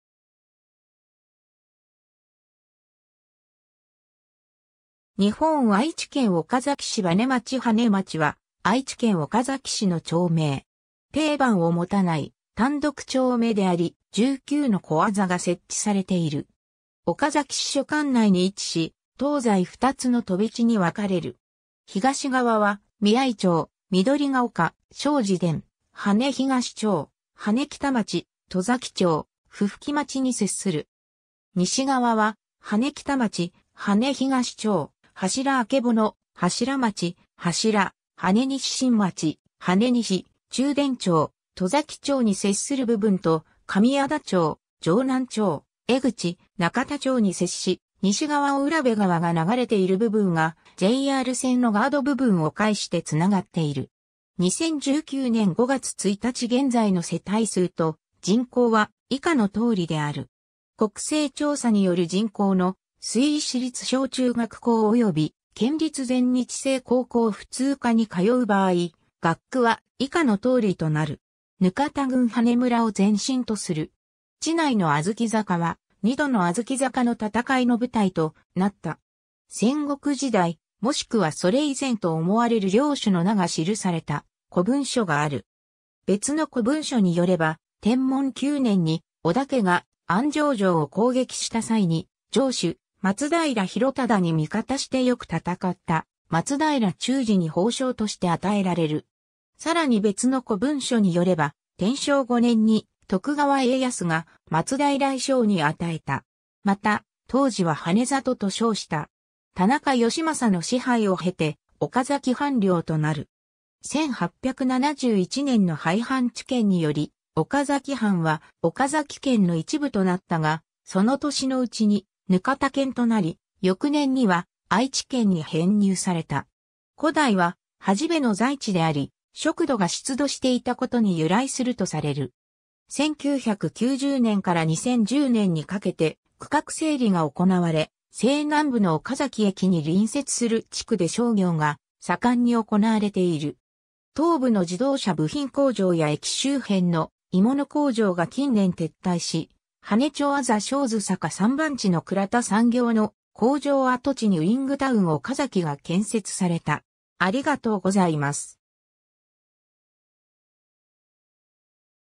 羽根町 (岡崎市)
岡崎市の町・字
日本 」 愛知県 」 岡崎市 」 羽根町 (岡崎市) 羽根町（はねちょう）は、愛知県岡崎市の町名。
丁番を持たない単独町名であり、19の小字が設置されている。
 岡崎支所管内に位置し、東西2つの飛地に分かれる。
東側は美合町・緑丘・庄司田・羽根東町・羽根北町・戸崎町・不吹町に接する。
西側は羽根北町・羽根東町・柱曙（一点だけ）・柱町・柱・羽根西新町・羽根西・中田町・戸崎町に接する部分と、上和田町・城南町・江口・中田町に接し、西側を占部川が流れている部分（羽根町字五反畑）がJR線のガード（JR羽根ガード）部分を介して繋がっている。
 2019年（令和元年）5月1日現在の世帯数と人口は以下の通りである。
 国勢調査による人口の推移 市立小・中学校および県立全日制高校普通科に通う場合、学区は以下の通りとなる。
 額田郡羽根村を前身とする。
 地内の小豆坂は、二度の小豆坂の戦いの舞台となった。
戦国時代、もしくはそれ以前と思われる領主の名が記された古文書がある。
別の古文書によれば、天文9年（1540年）に織田家が安祥城を攻撃した際に、城主松平広忠に味方してよく戦った松平忠次に褒賞として与えられる。
さらに別の古文書によれば、天正5年（1577年）に徳川家康が松平伊昌に与えた。
また、当時は羽（禰の左側が示）郷と称した。
田中吉政の支配を経て岡崎藩領となる。
 1871年（明治4年）の廃藩置県により岡崎藩は岡崎県の一部となったが、その年のうちに額田県となり翌年には愛知県に編入された。
 古代は土師部の在地であり、埴土が出土していたことに由来するとされる。
 1990年から2010年にかけて区画整理が行われ、西南部の岡崎駅に隣接する地区で商業が盛んに行われている。
東部の自動車部品工場や駅周辺の鋳物工場が近年撤退し、羽根町字小豆坂3番地のクラタ産業の工場跡地にウイングタウン岡崎が建設された。